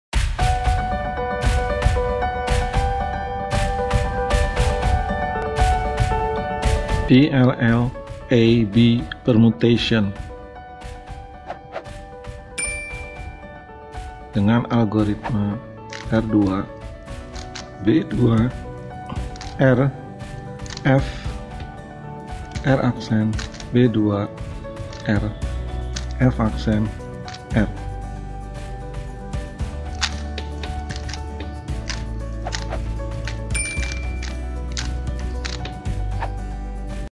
RUMUS Ab Permutation RUBIK 3x3 bagi PEMULA Rubik's Cube
Rumus [Algoritma] PLL Ab Permutation bagi PEMULA (Rubik 3x3)

Video ini merupakan salah satu video dari rangkaian latihan rumus (algoritma) metode CFOP (Rubik 3x3).

Video sebelumnya ...

Video selanjutnya ...